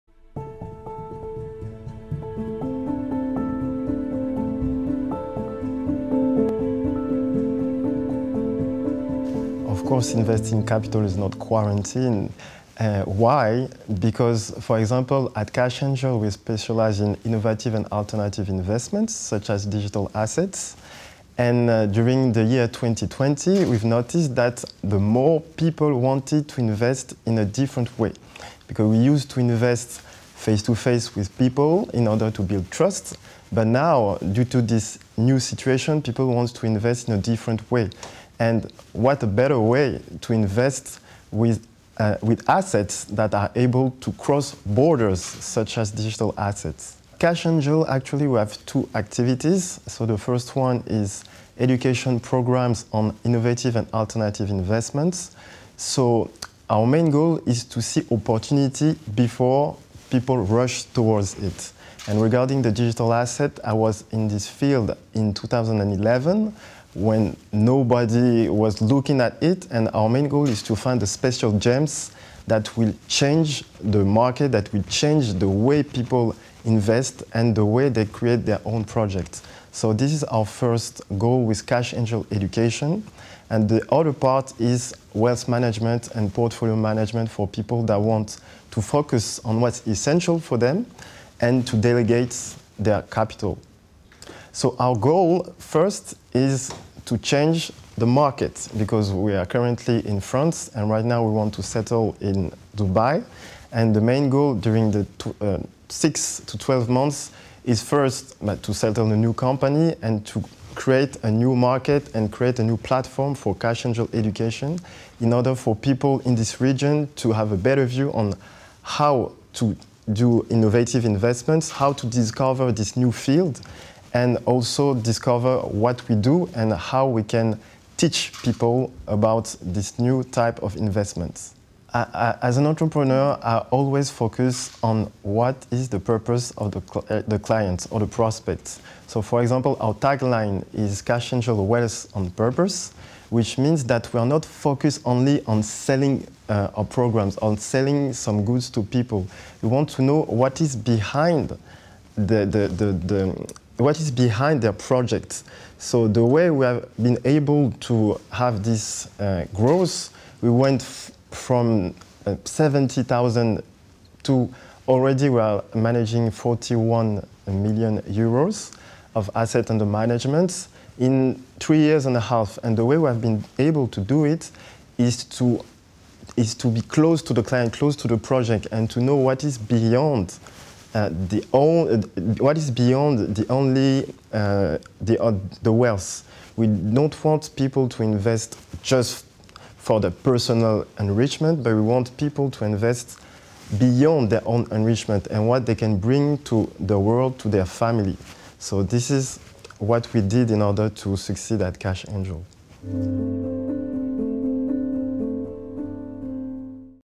Xolali Zigah, Founder & Chairman, Cash Angel
Under the High Patronage of The Ministry of Economy
In Presence of His Excellency Dr Thani Ahmed Al Zeyoudi, Minister of State for Foreign Trade, UAE
Sir Anthony Ritossa's 14th Global Family Office Investment Summit

14th Conference, Dubai - March 28-30, 2021